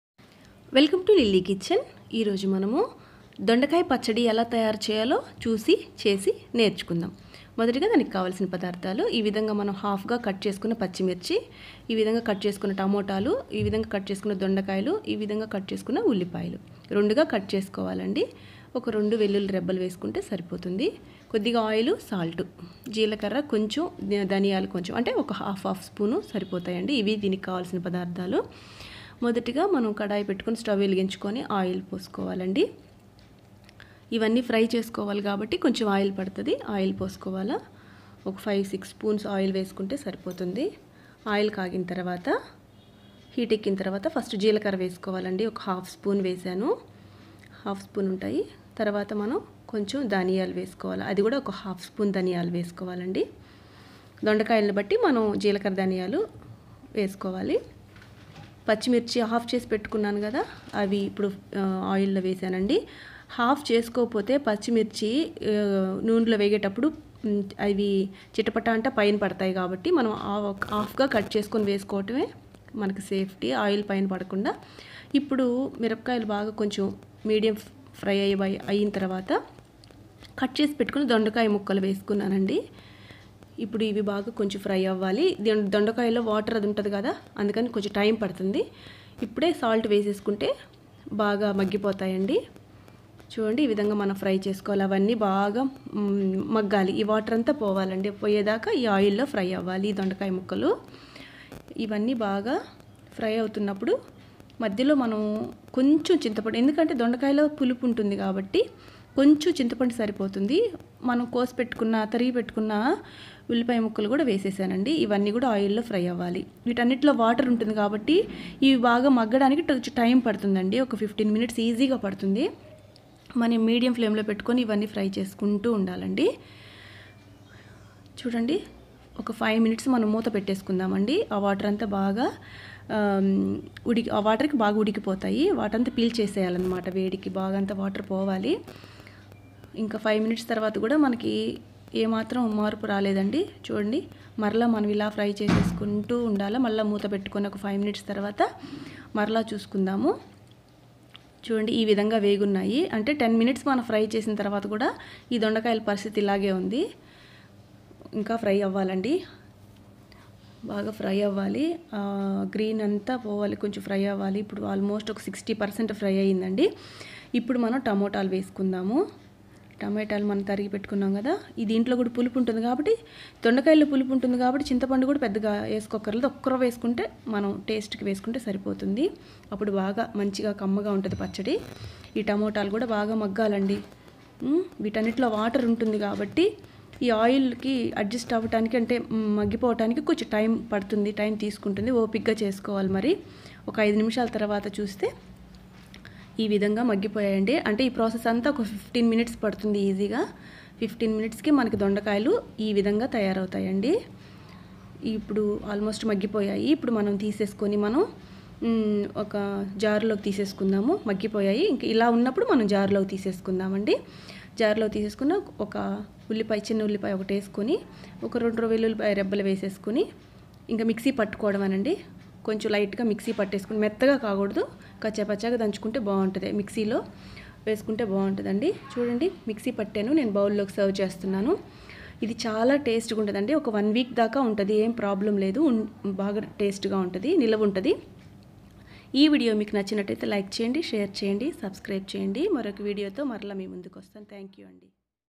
Tinda Donda pachadi|| Home made Healthy Tinda chetny|| Easy & tasty Donda pachadi
hi friends today iam going to show you healthy home made easy tasty tinda chetny